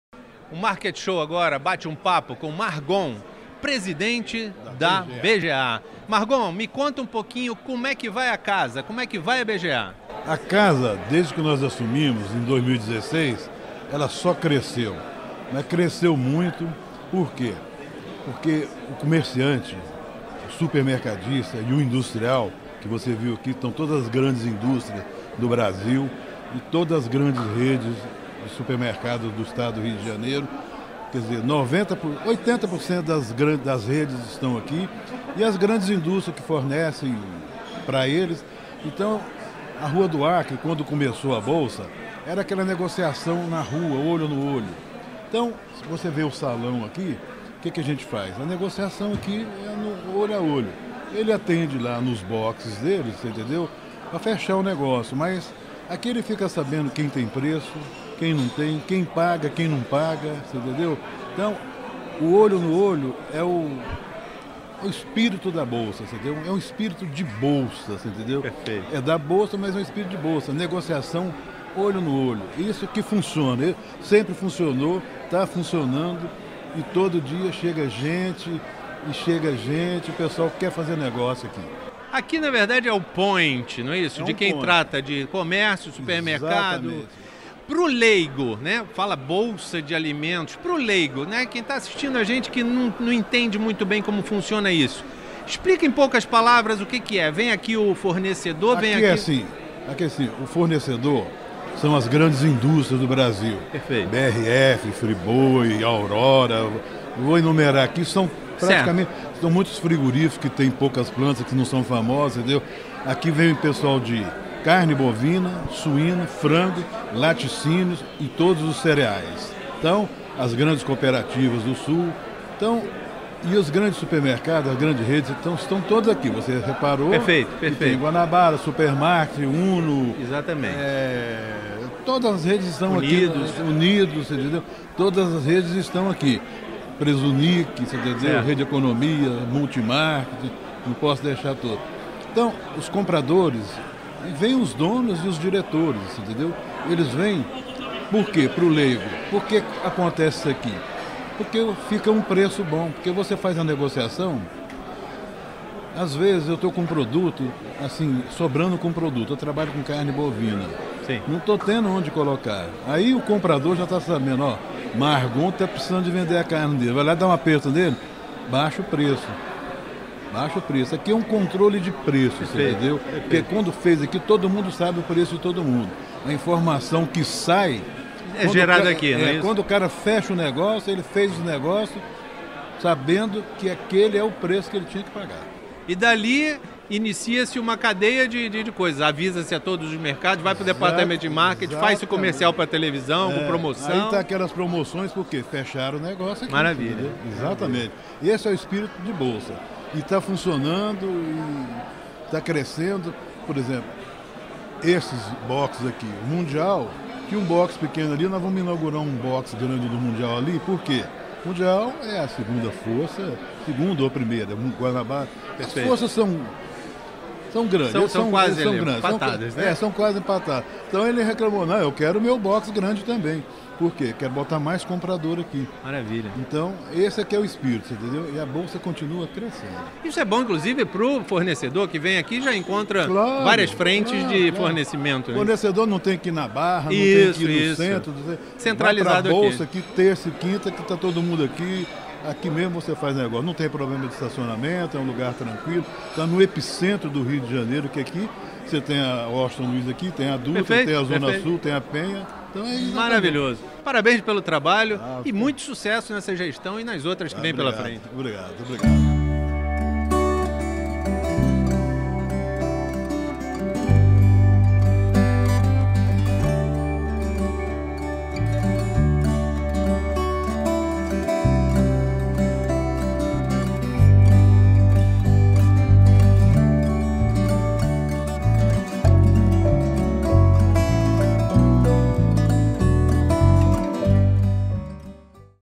BGA - Humberto Margon BGARJ
Entrevista com Humberto Margon, presidente da BGARJ (Bolsa de Gêneros Alimentícios do Estado do Rio de Janeiro).

Reportagem: Marcelo Ribeiro
Imagens: Marcio de Andrade
Produção: Robson Barreto
Realização: Rib Art Produções